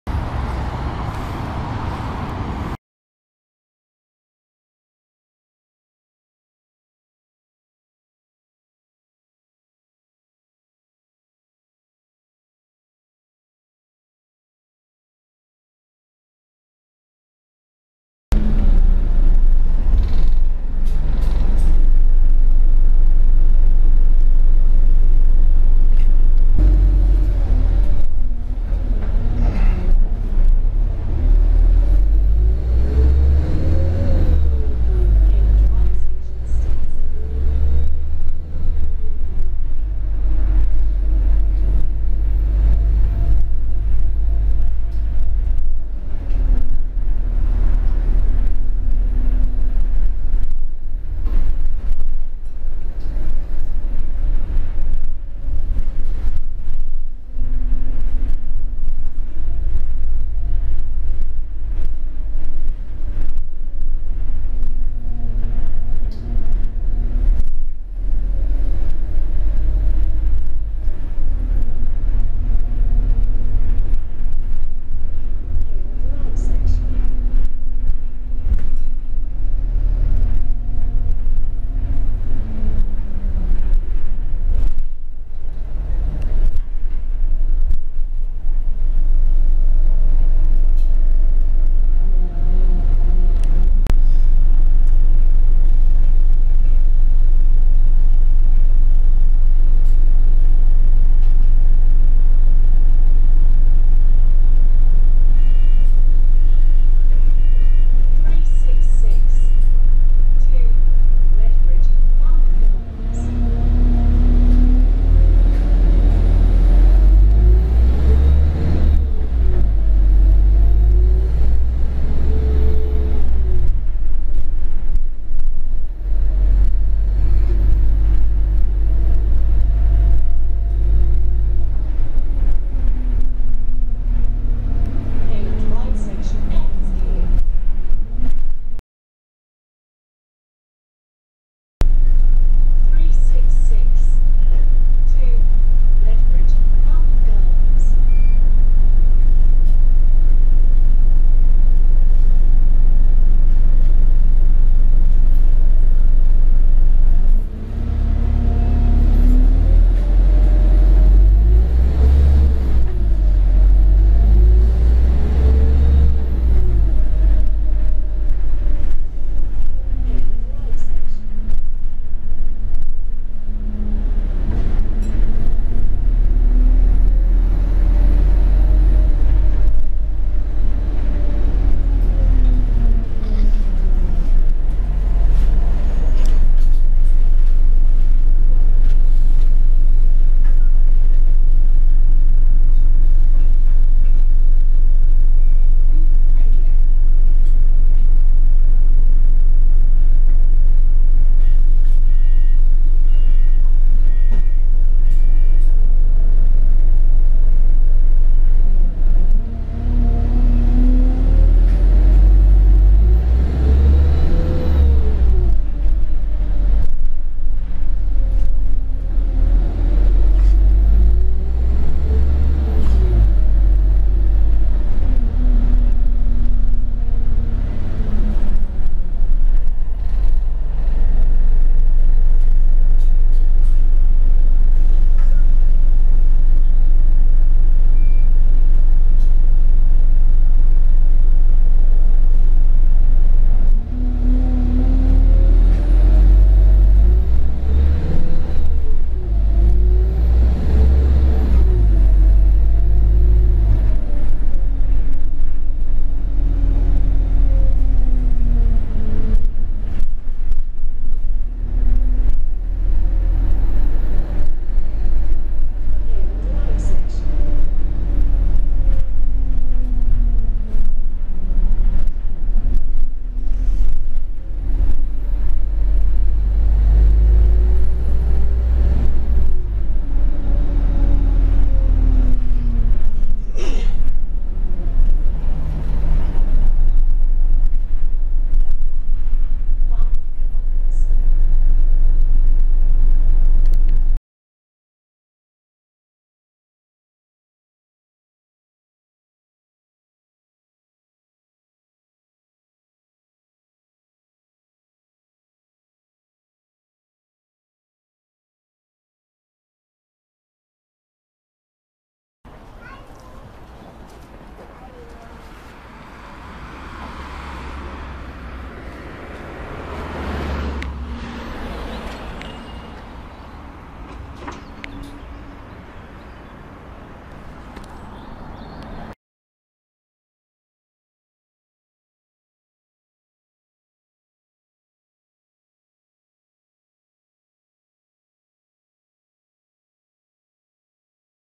Enviro 200 Euro 6 Go Ahead London Blue Triangle SE280 YX65RRY Short Journey on the Route 366
Route 366 from Redbridge Station to Redbridge Farmouth Gardens. 05/05/2023